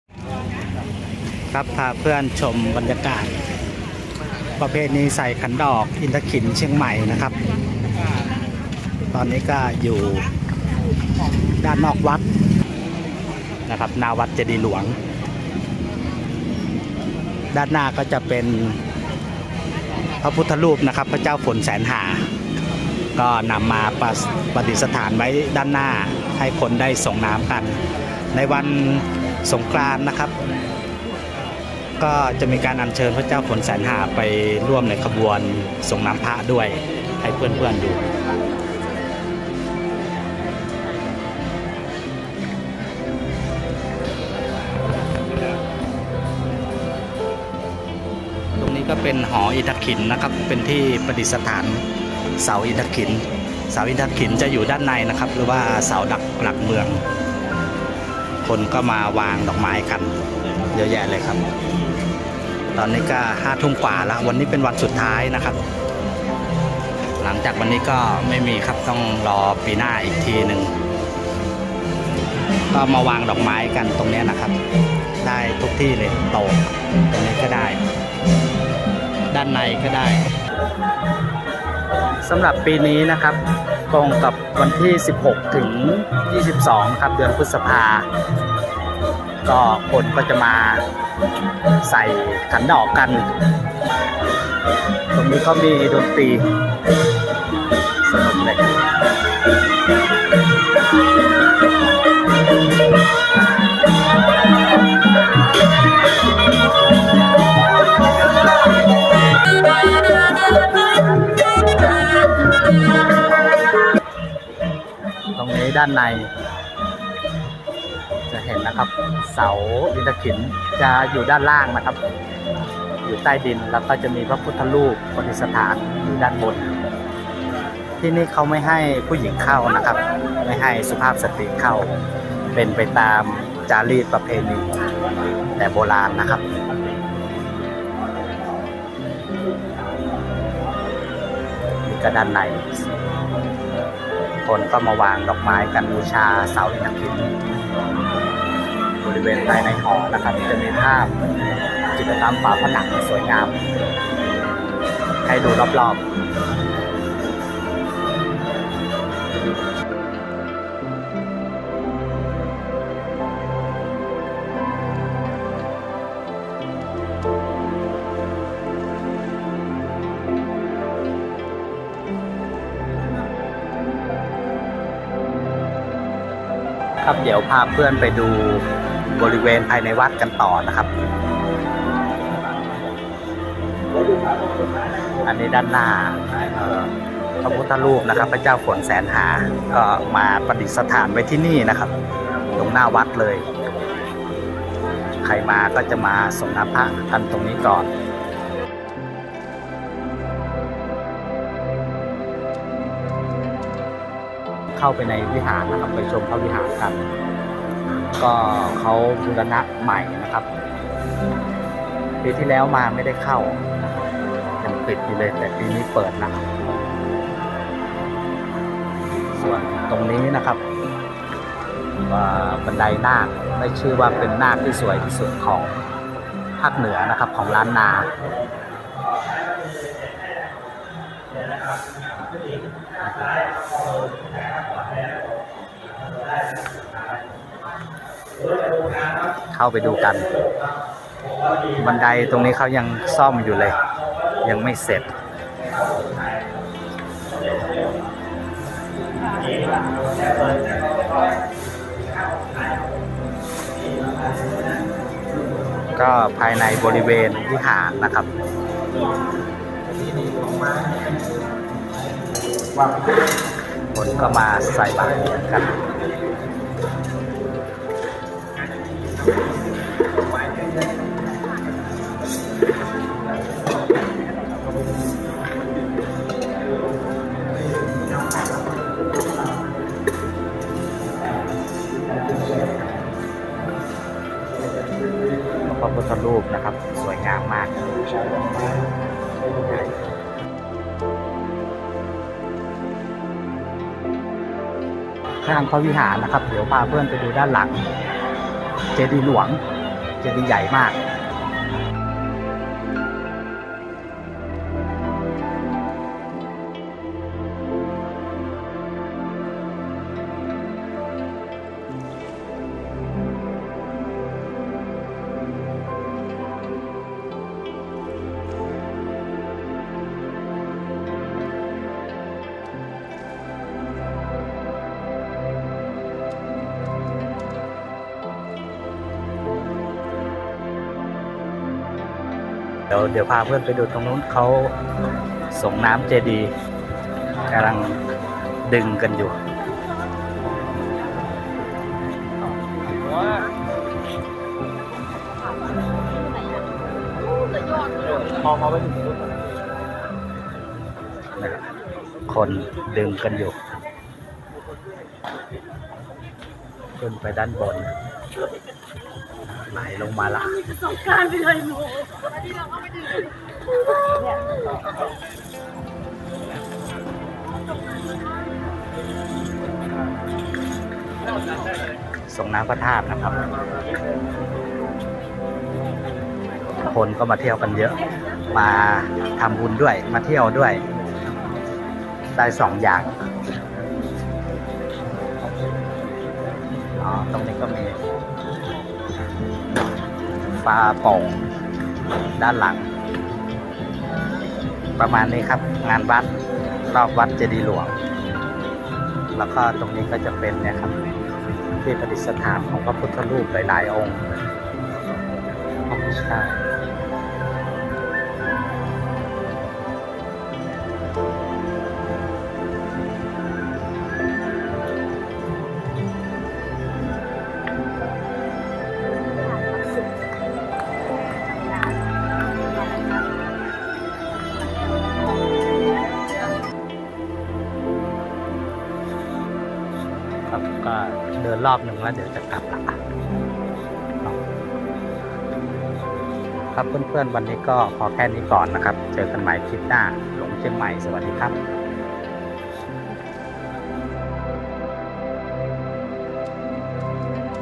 อินทขินเชียงใหม่ ประเพณีใส่ขันดอกบูชาเสาหลักเมือง2566
พาชมบรรยากาศประเพณีใส่ขันดอกบูชาเสาหลักเมืองเชียงใหม่ประจำปีพ.ศ 2566 จัดขึ้นตั้งแต่วันที่ 16 ถึงวันที่ 22 พฤษภาคม ณ.วัดเจดีย์หลวงวรวิหารจังหวัดเชียงใหม่ เป็นประเพณีที่เชื่อว่าจะทำให้บ้านเมืองพ้นภัยและเกิดความร่มเย็นเป็นสุขเจริญรุ่งเรือง ภายในวัดจะมีจุดใส่ขันดอกรอบบริเวณเสาอินทขิลและบริเวณรอบพระธาตุเจดีย์หลวง ประเพณีใส่ขันดอกอินทขิล เป็นประเพณีที่งดงามและทำสืบต่อกันมาจนถึงปัจจุบัน